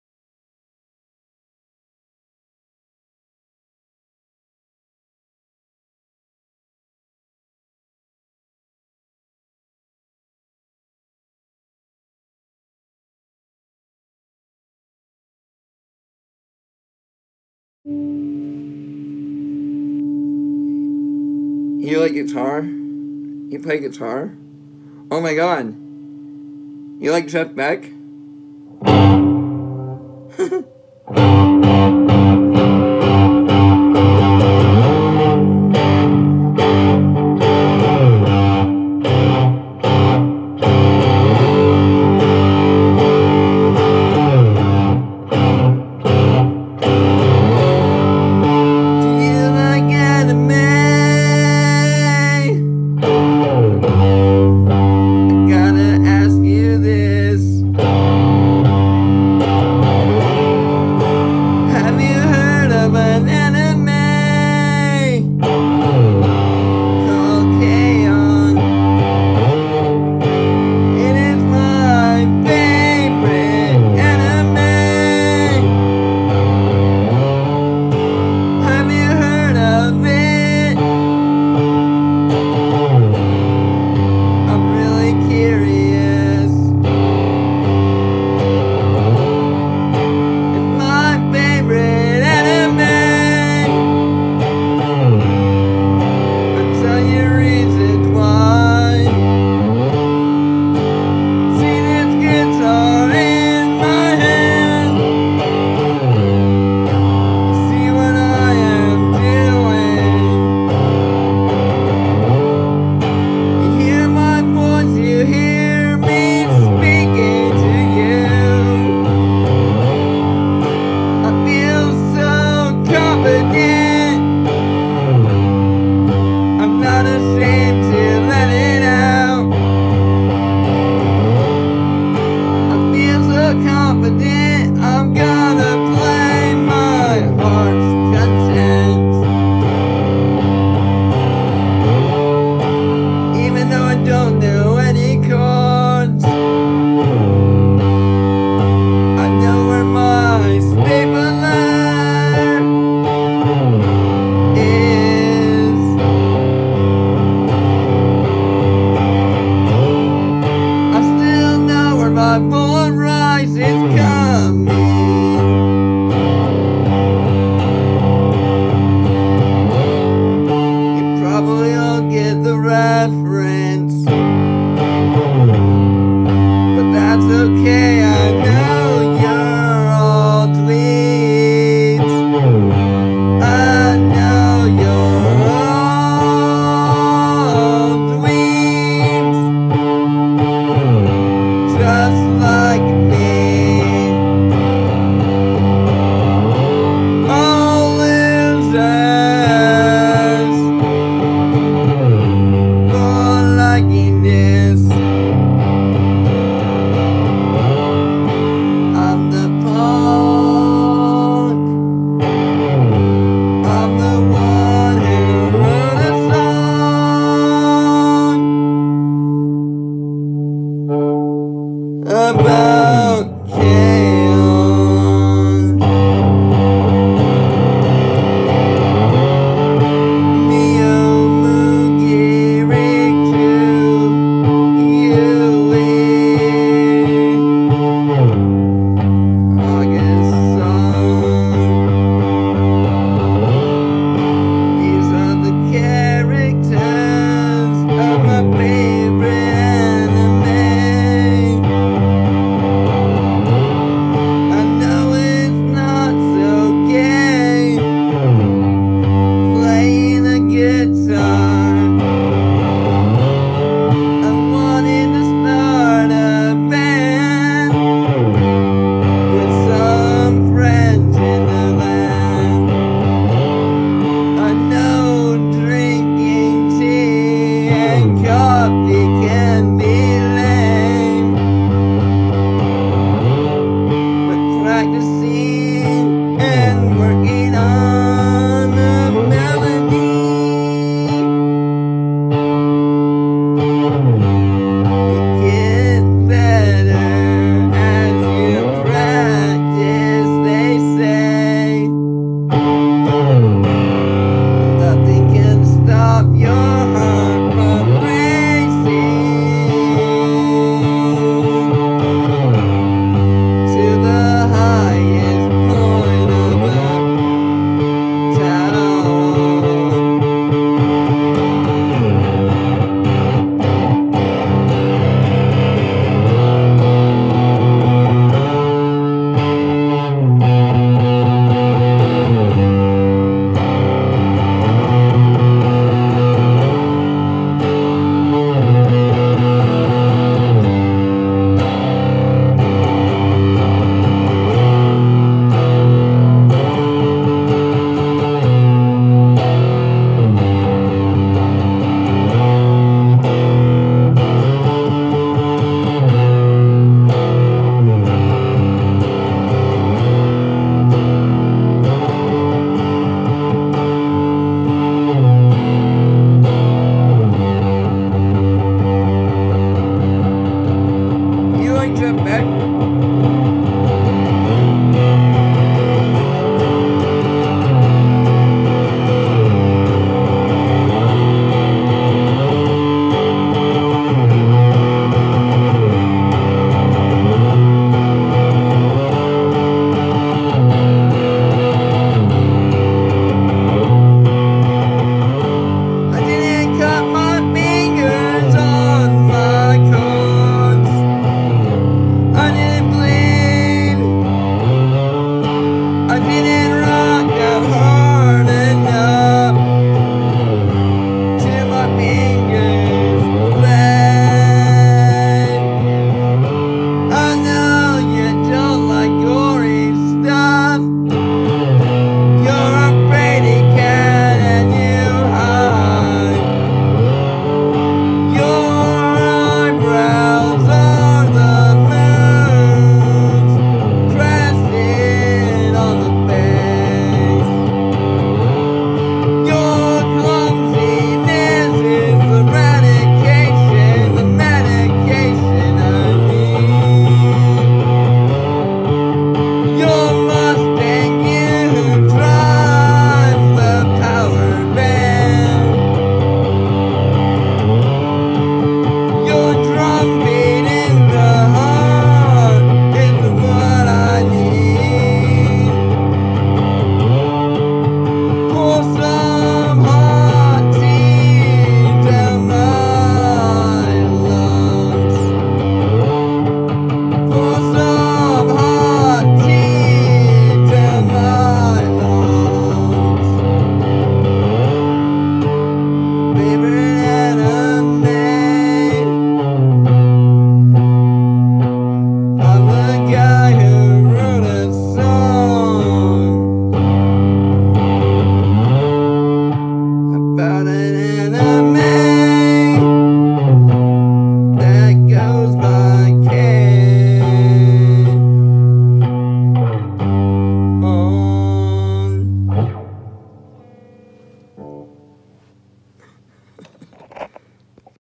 You like Jeff beck k on! Joke nowadays
Here’s my song lyrics about my guitar inspiration from watching anime series called Kon!